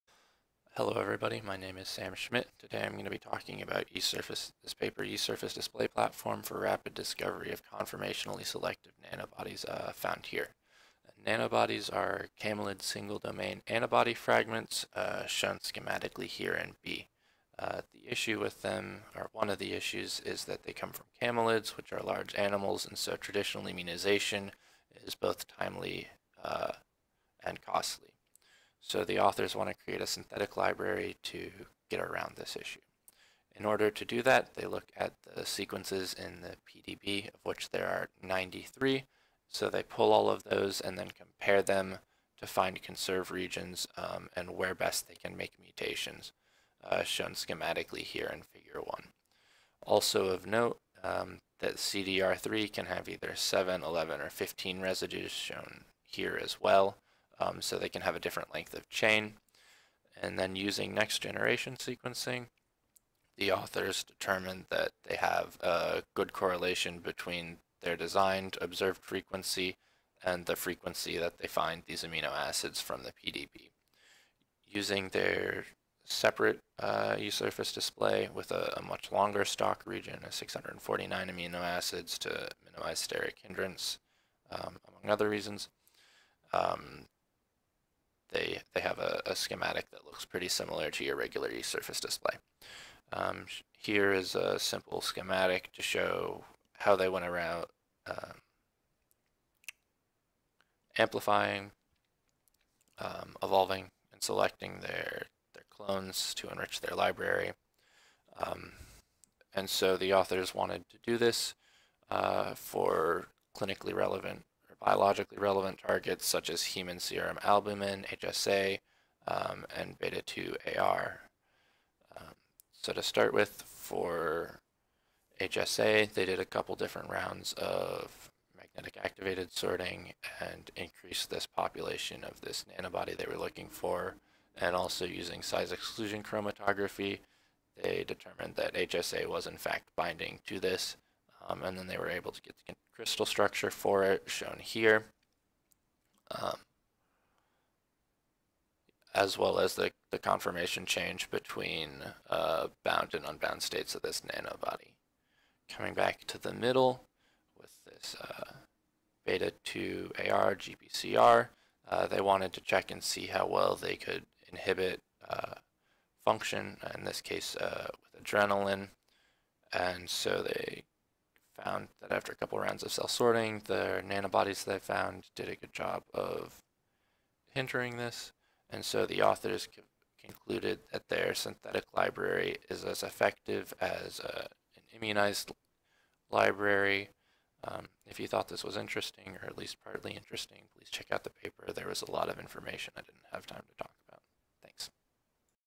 Yeast surface display platform for nanobody discovery, McMahon-Kruse, Nat Struct Mol Biol (2018)
Sam Schmidt Presents:

C. McMahon et al., “Yeast surface display platform for rapid discovery of conformationally selective nanobodies,” Nat. Struct. Mol. Biol., vol. 25, no. 3, pp. 289–296, 2018, doi: 10.1038/s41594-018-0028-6.